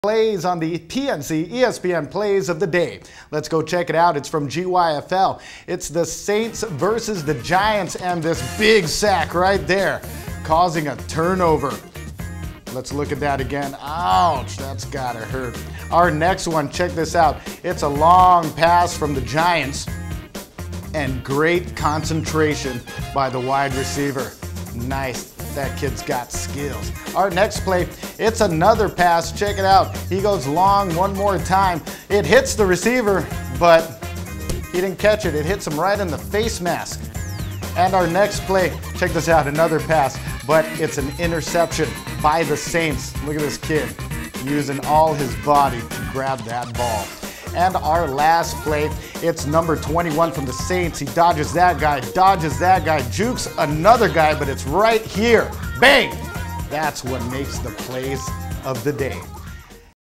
Plays of the Day: October 6, 2008
top plays from the Guam Youth Football League:  Saints vs. Giants